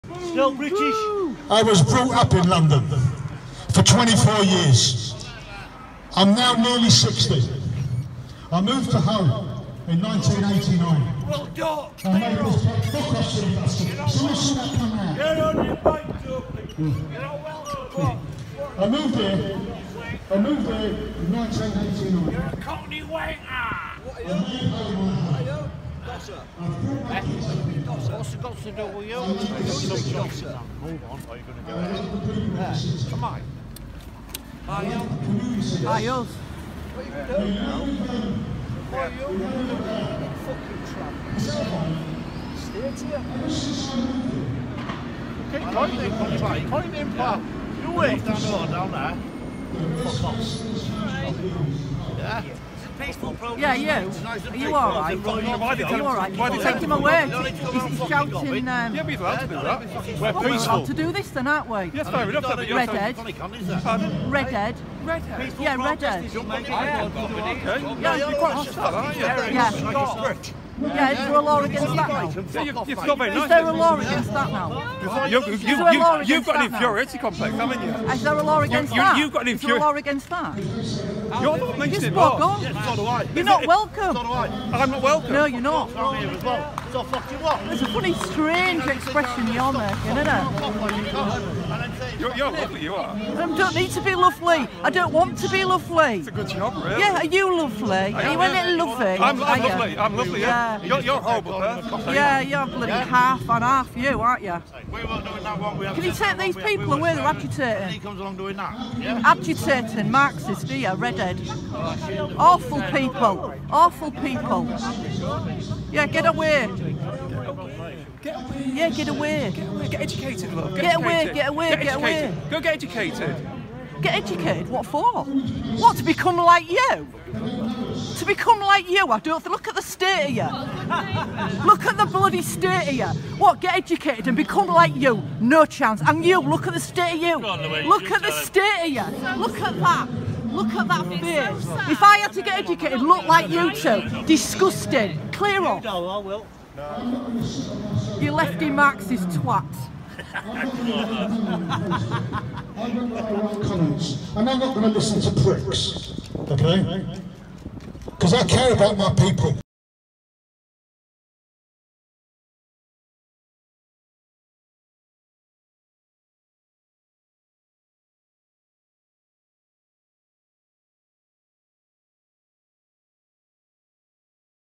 Hulls Marxist Infestation Get Told To Do One As They Ugly !!
Hull is full of Marxists these two melts think they are the saviours of Hull
Got told to do one

Support the push back if you can after fuel all donations will go to less fortunate children of god